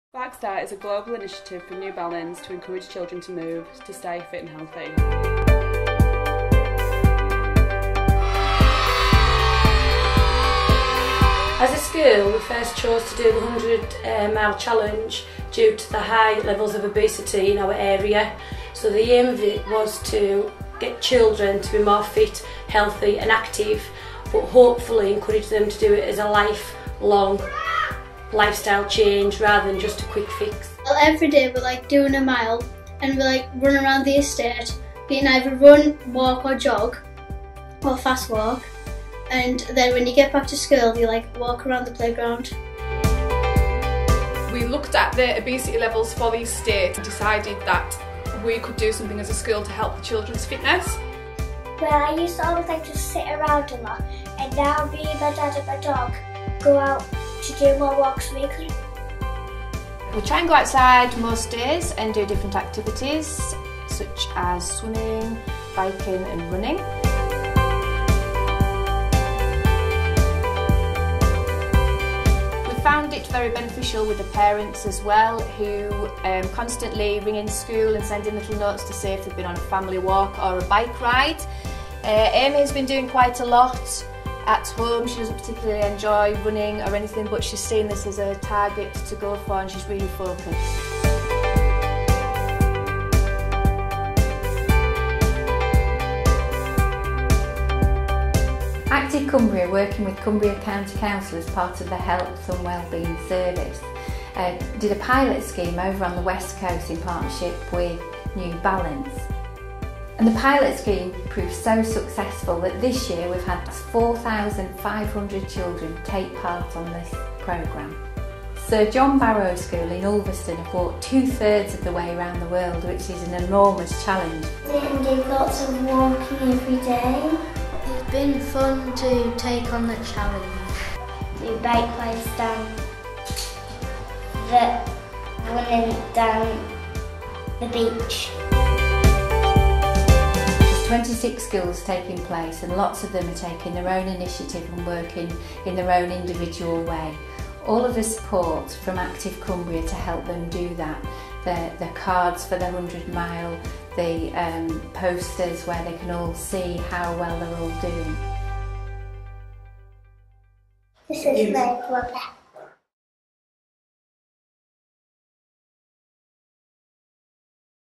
100 Mile Challenge
This video is an overview of the Active Cumbria's 100 Mile Challenge with New Balance (Flimby) and the SparkStart Global Initiative.

26 schools & 4500 children have taken part from across Cumbria in the academic year 2015/16

Video features
Ewanrigg Junior School, Maryport.
Shap Primary School, Shap, and
Sir John Barrow School in Ulverston.